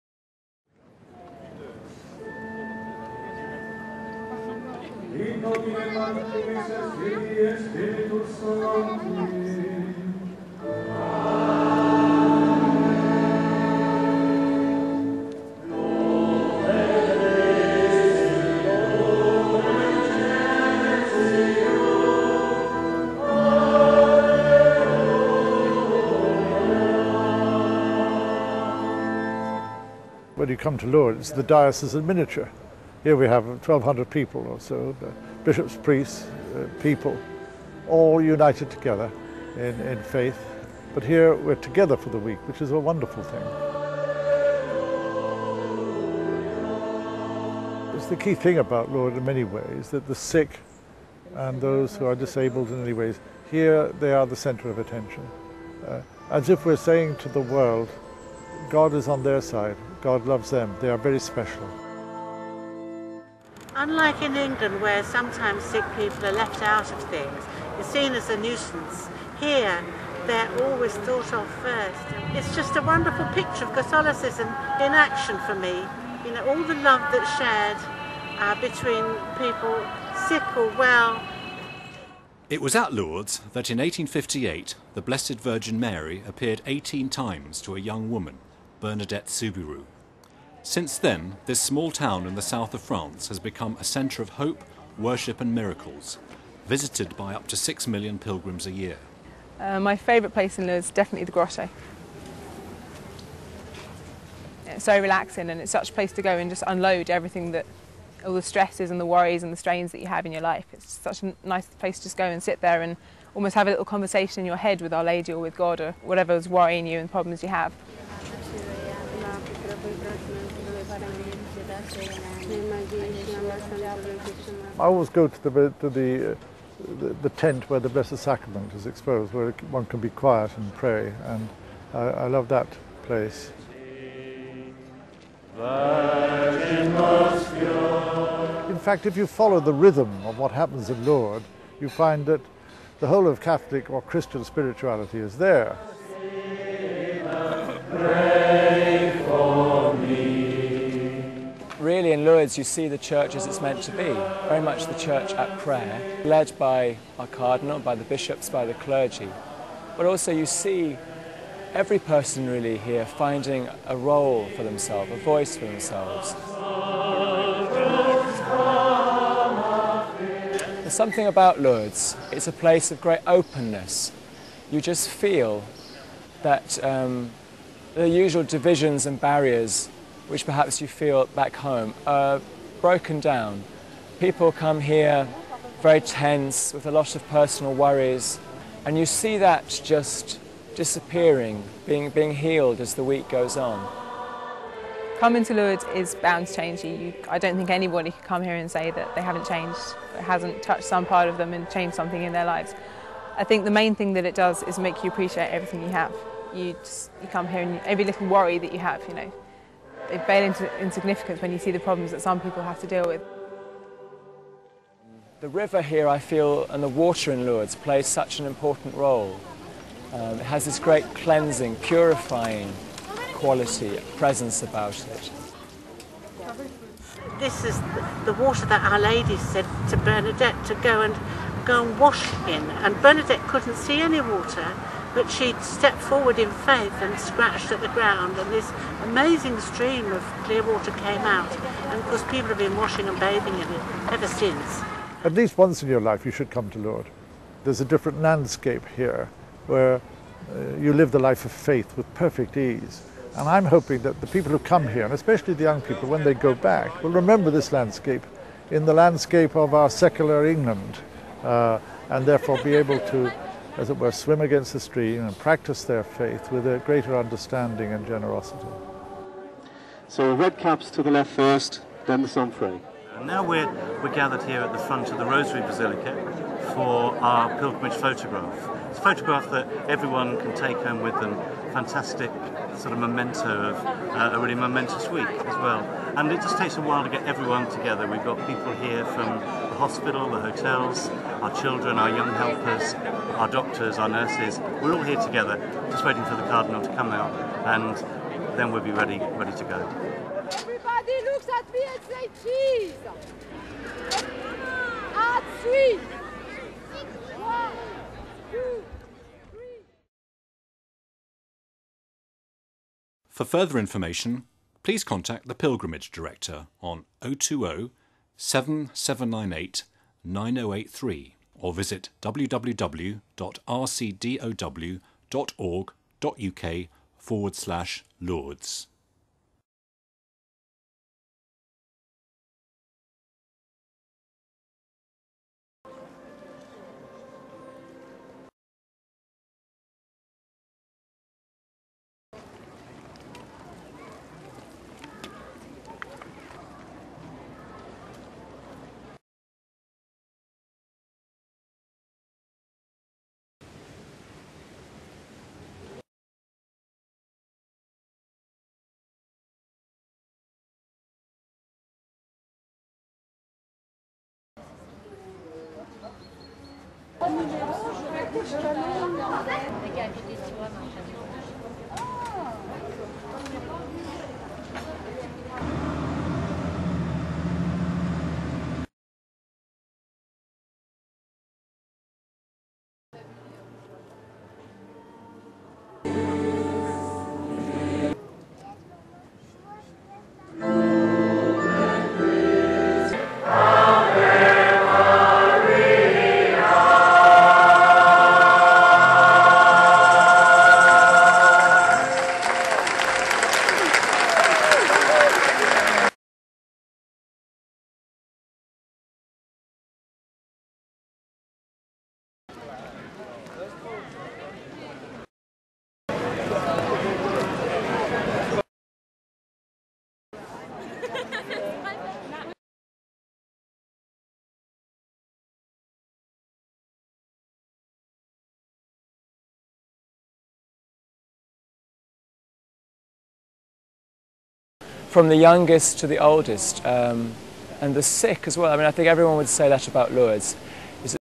Lourdes -- a Place of Pilgrimage
Faith and hope as pilgrims from London explore the meaning of Lourdes